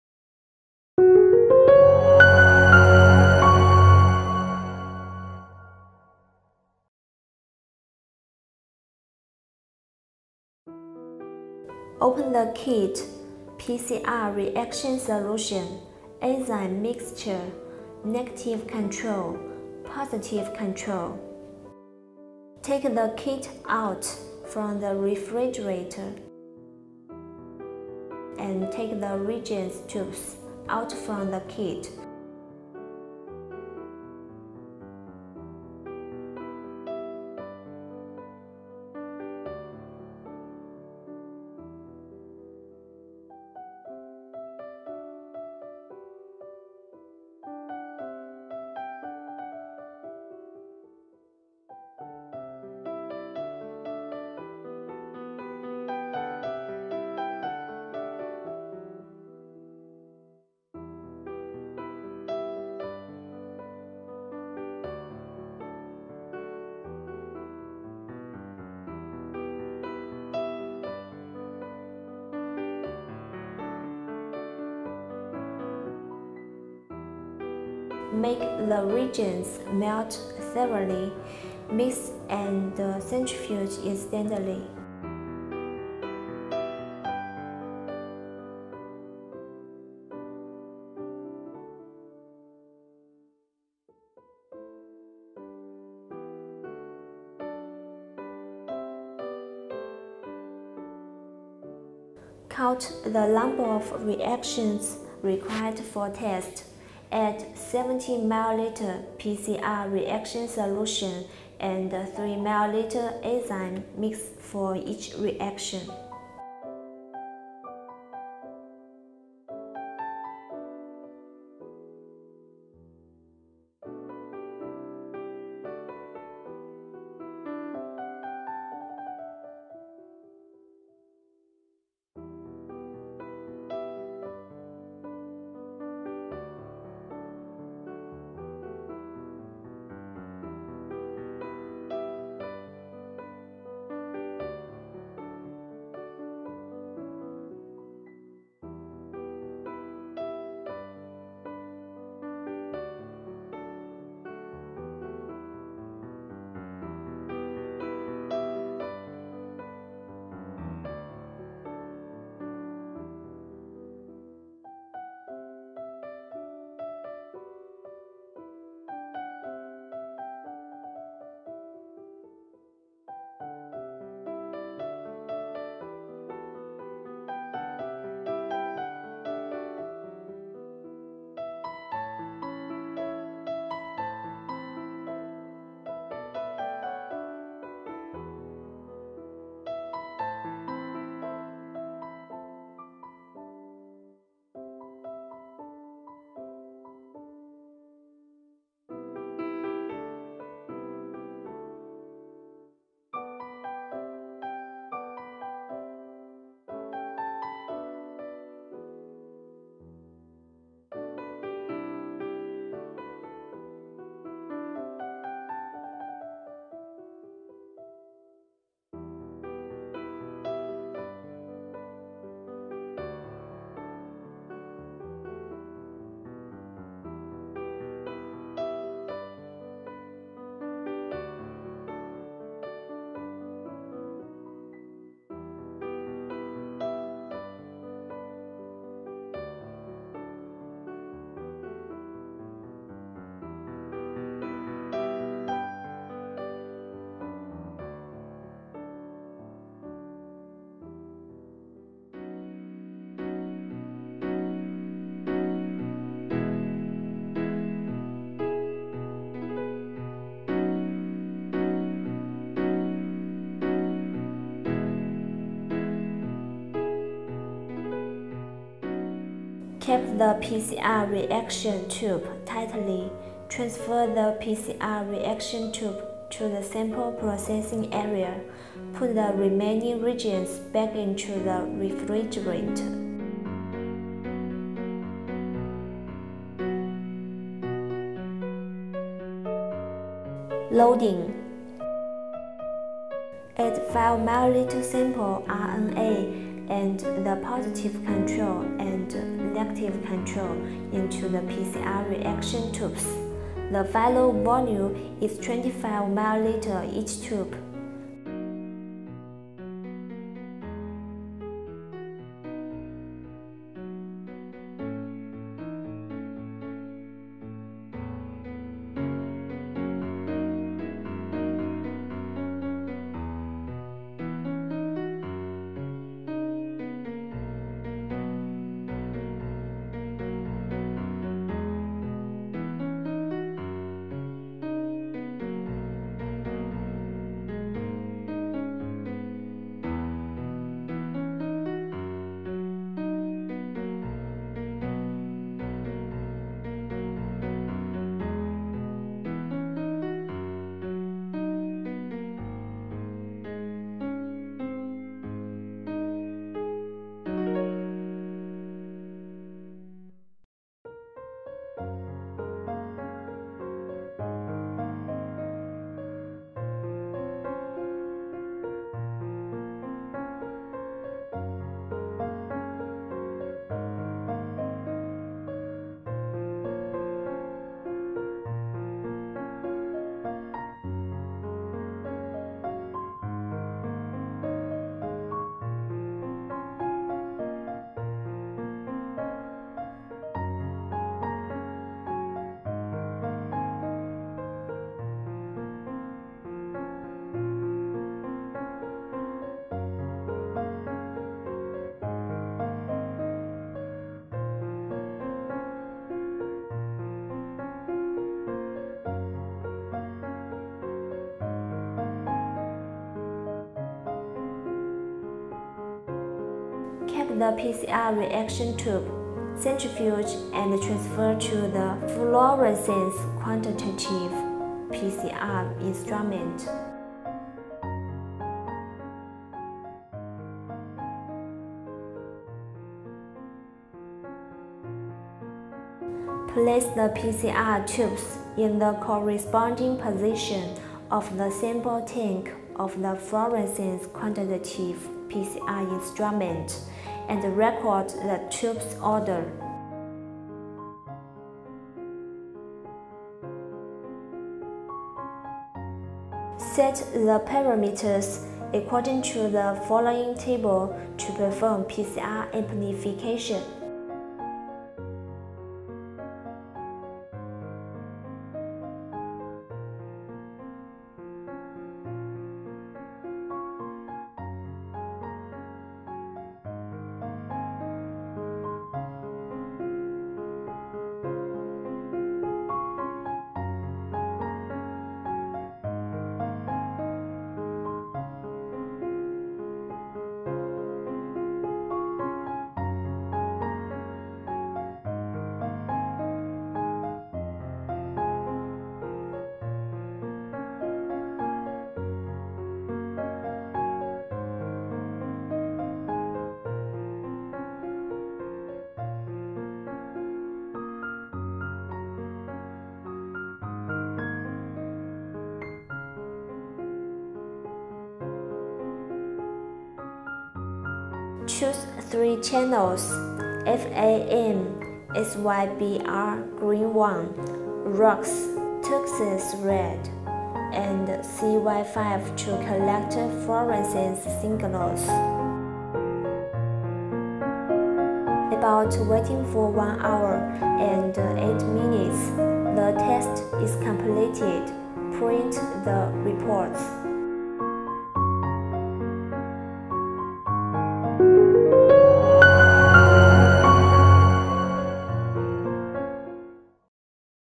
New Coronavirus COVID-19 Nucleic Acid Detection Kit Fluorescent RT PCR Method
New Coronavirus test kit, COVID-19 detection kit, fluorescent RT-PCR method nucleic acid detection kit operating instruction video - Hunan Runmei Gene Technology Co., Ltd.